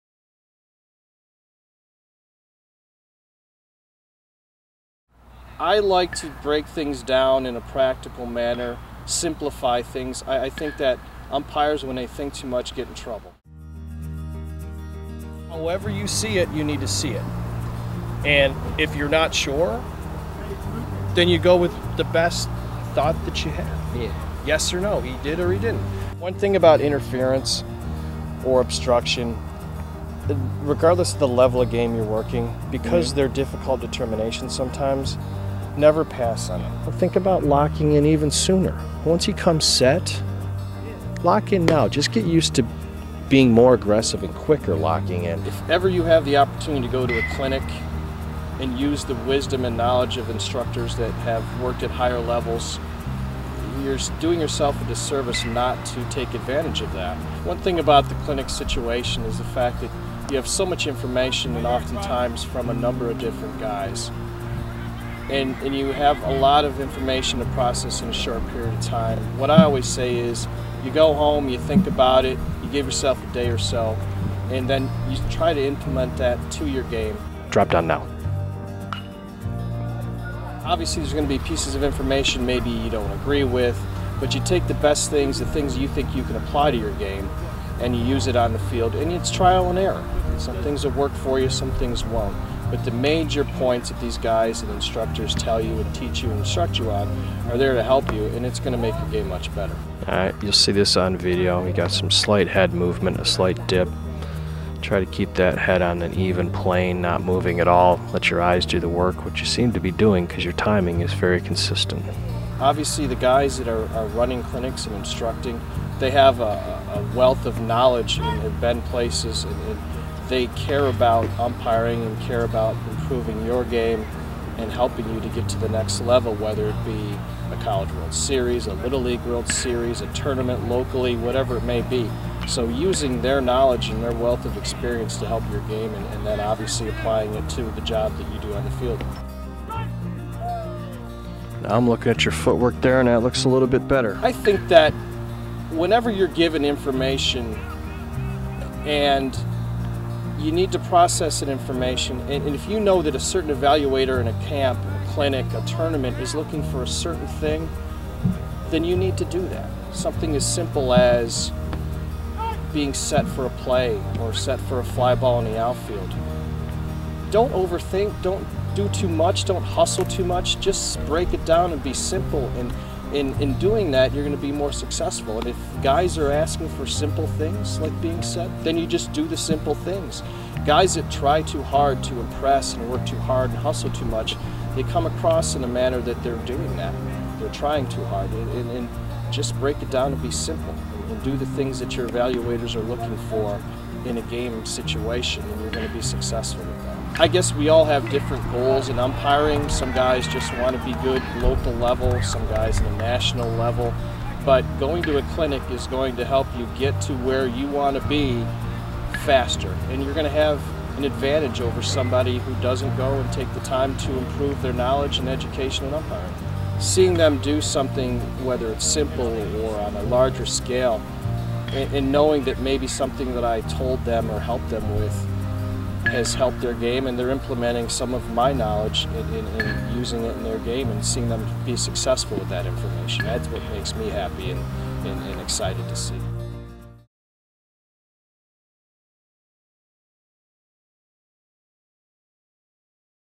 NJSAB Live Game Evaluations Video Trailer, Featuring Chris Marshall
As part of The NJSAB's commitment to umpire development, NCAA Regional Umpire, Chris Marshall recently evaluated four games and provided valuable insight to the NJSAB staff umpires working. In addition to the immediate day of game feedback, each umpire received a comprehensive video file featuring extensive commentary from Chris.